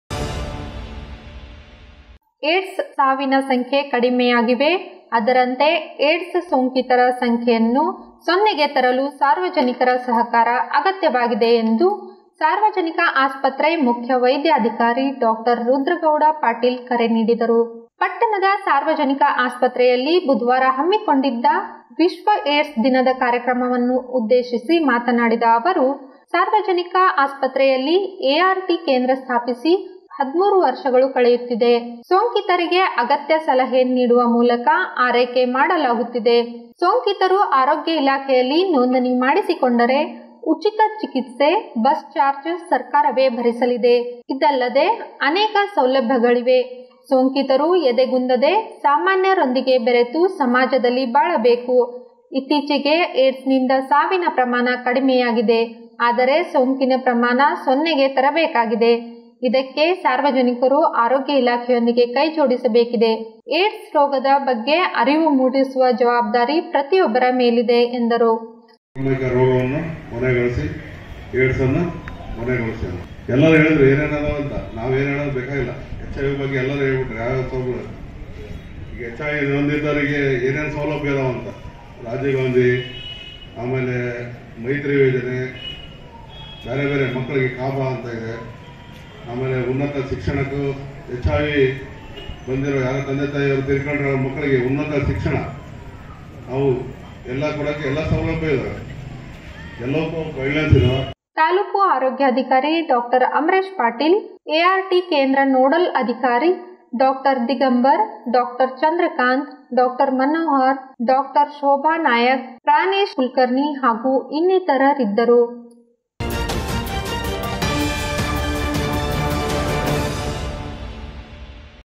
ಏಡ್ಸ್ ಸೊಂಕಿತರ ಸಂಖ್ಯೆ ಸೊನ್ನೆಗೆ ತರಲು ಸಹಕಾರ ಅಗತ್ಯ Lingasugur News SLVC NEWS 01 12 2021
SLVCChannel